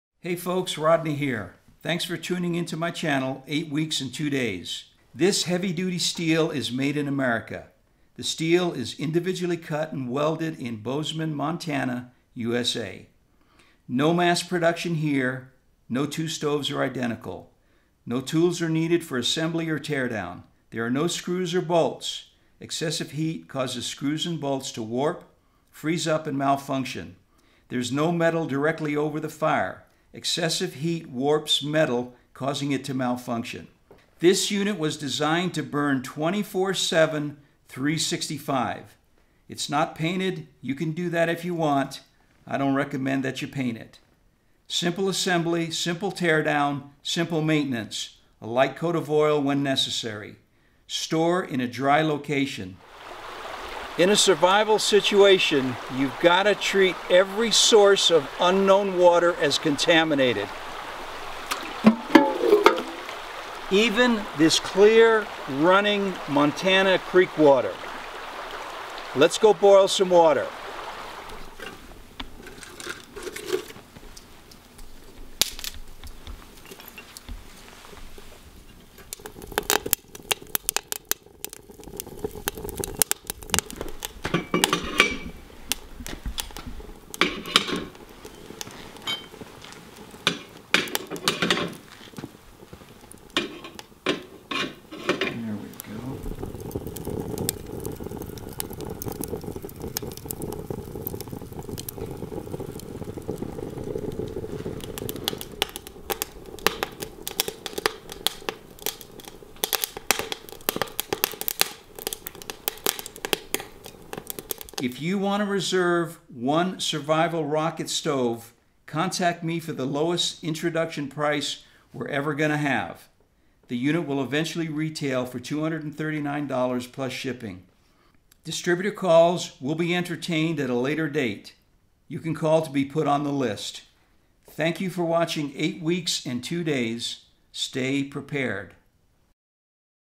Rocket Stove Design for Survival - Part 2
View the detailed description and fired up demonstration of this revolutionary designed Survival Rocket Stove.